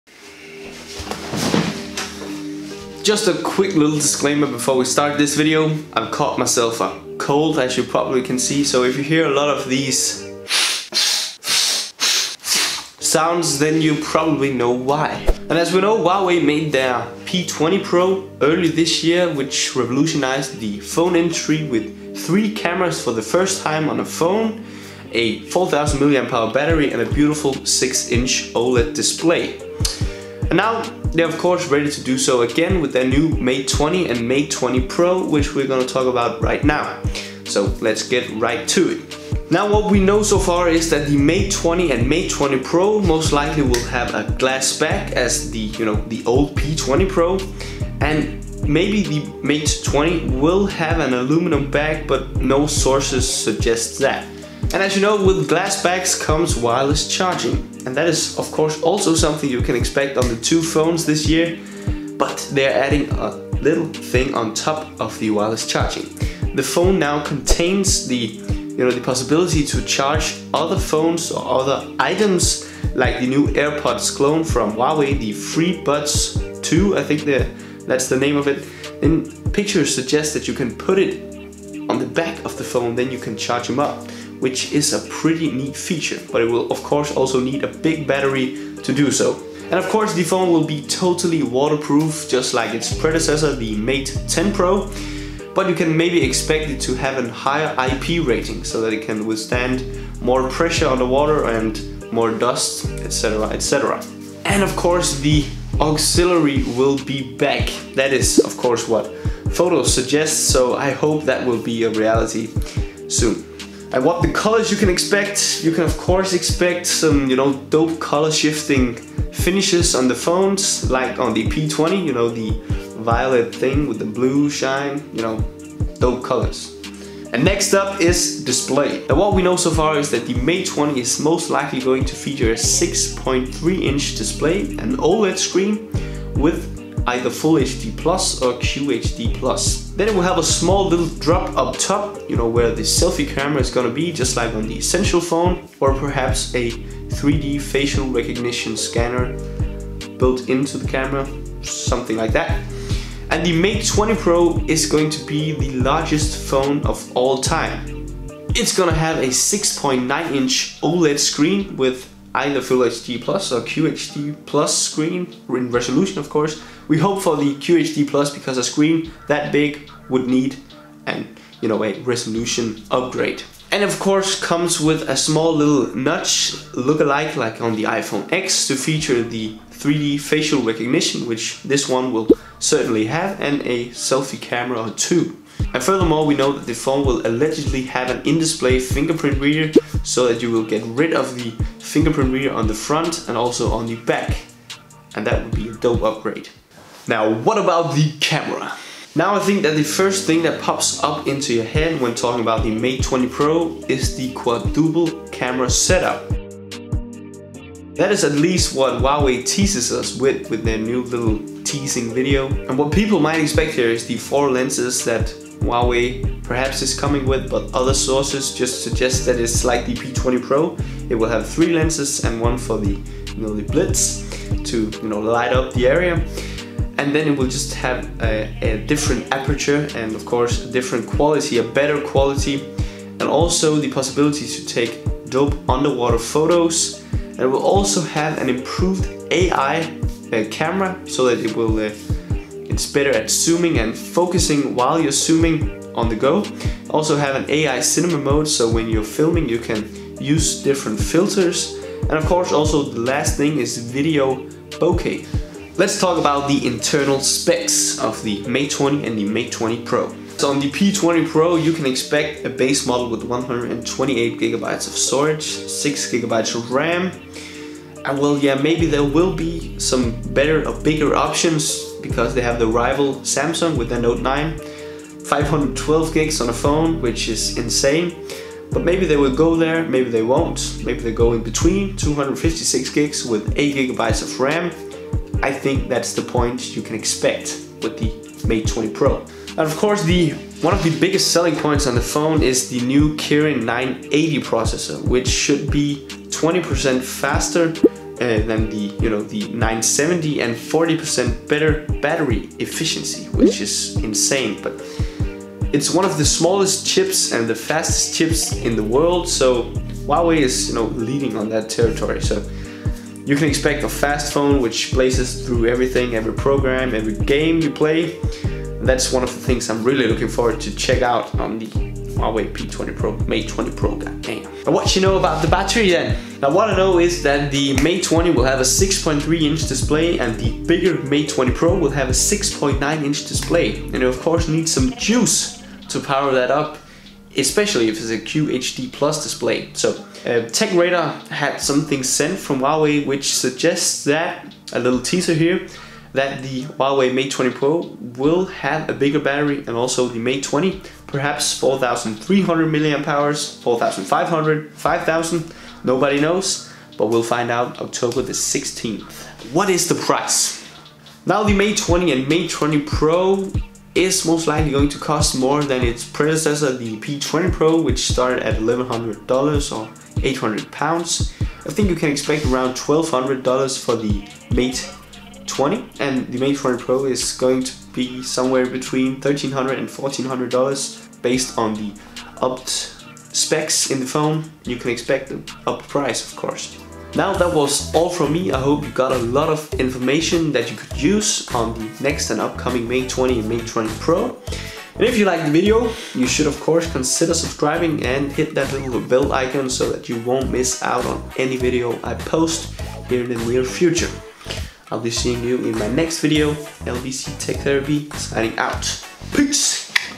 Huawei Mate 20 (Pro) | Why is this phone so revolutionary?
The Huawei Mate 20 and Mate 20 Pro is coming, and I can't wait to see what they will show us October the 16th in London! So to be a little bit ahead of the show, I searched the internet to find all the latest details about the two flagships Huawei is revealing next month.